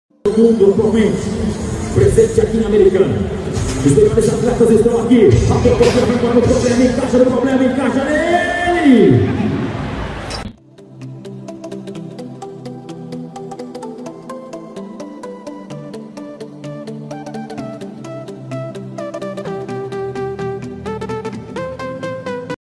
KAIQUE PACHECO VS PROBLEMA CIA EURIPÍNHO SOLLO
rodeio de americana nota 0,00
imagens cinegrafista amador